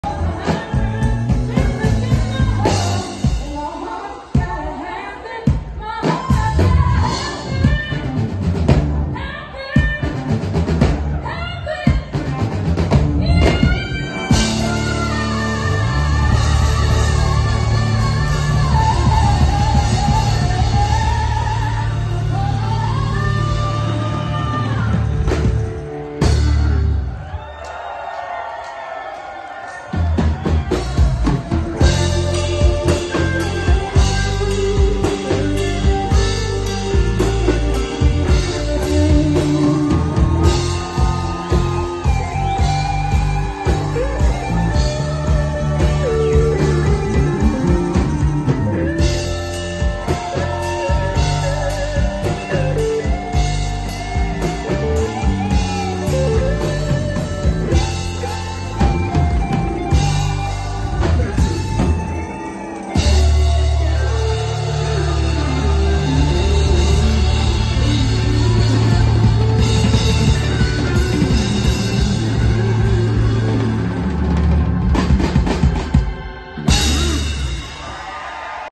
Wish Every Show End Like This…
Headlining Baton Rouge Blues Festival, amazing crowd, good vibes last night.